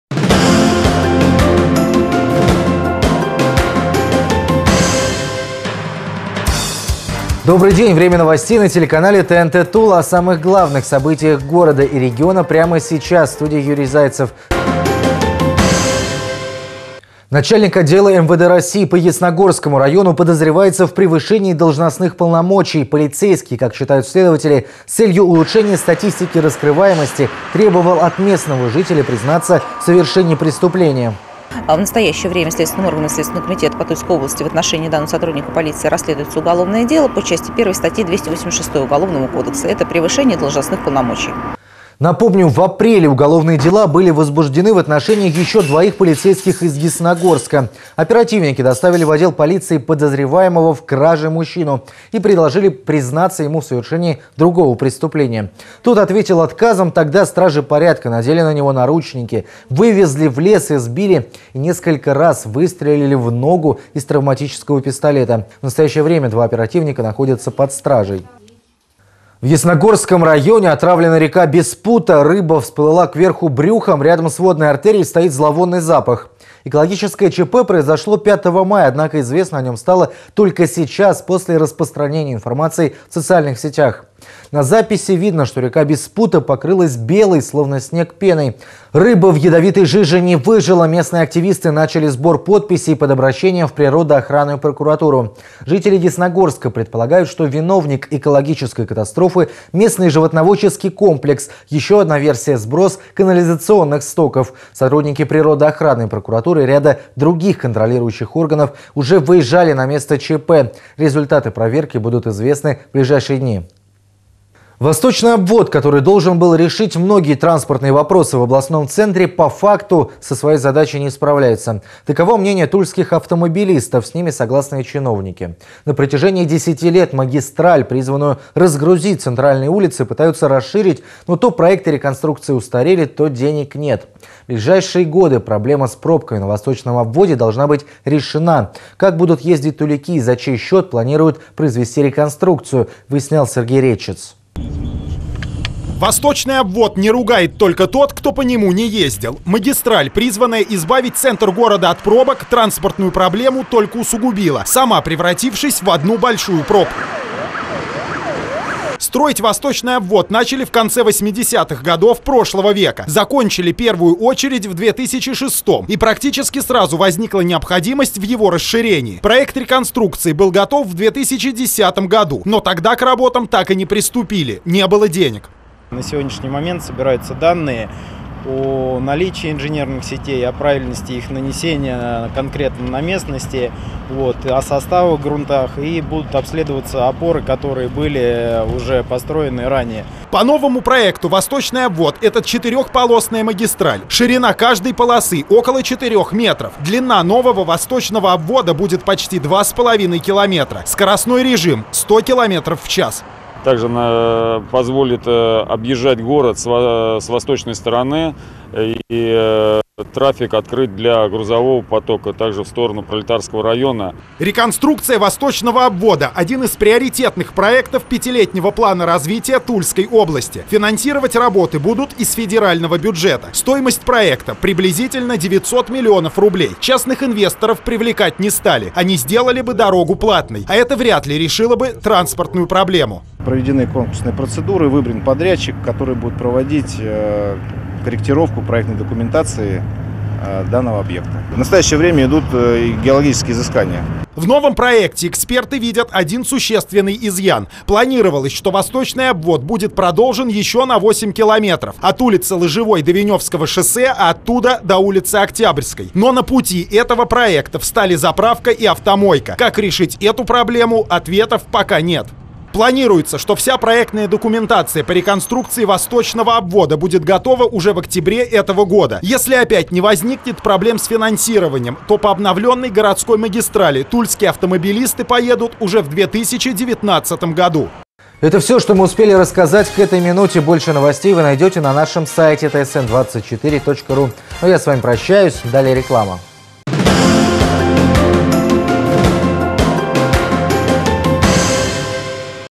Дневные новости ТСН24 от 11 мая 2017 года
Сегодня в выпуске:

Начальник отдела МВД России по Ясногорскому району подозревается в превышении должностных полномочий. Полицейский, как считают следователи, с целью улучшения статистики раскрываемости требовал от местного жители признаться в совершении преступления. Напомним, в апреле уголовные дела были возбуждены в отношении еще двоих полицейских из Ясногорска. Оперативники доставили в отдел полиции подозреваемого в краже мужчину и предложили ему признаться в совершении преступления. Тот ответил отказом. Тогда стражи порядка надели на него наручники, вывезли в лес, избили и несколько раз выстрелили в ногу из травматического пистолета. Мужчина под пытками сознался в совершении преступления, после чего полицейские оформили явку с повинной. В настоящее время два оперативника находится под стражей.

В Ясногорском районе отравлена река Беспута. Рыба всплыла кверху брюхом, рядом с водной артерией стоит зловонный запах. На записи видно, что река Беспута покрылась белой, словно снег, пеной. Рыба в ядовитой жиже не выжила. Местные активисты начали сбор подписей под обращением в природоохранную прокуратуру. Жители Ясногорска предполагают, что виновник экологической катастрофы – местный животноводческий комплекс. Еще одна версия – сброс канализационных стоков. Сотрудники природоохранной прокуратуры и ряда других контролирующих органов уже выезжали на место ЧП. Результаты проверки будут известны в ближайшие дни.

Восточный обвод, который должен был решить многие транспортные вопросы в областном центре, по факту со своей задачей не справляется. Таково мнение тульских автомобилистов. С ними согласны и чиновники. На протяжении 10 лет магистраль, призванную разгрузить центральные улицы, пытаются расширить, но то проекты реконструкции устарели, то денег нет. В ближайшие годы проблема с пробками на восточном обводе должна быть решена. Как будут ездить туляки, и за чей счет планируют провести реконструкцию?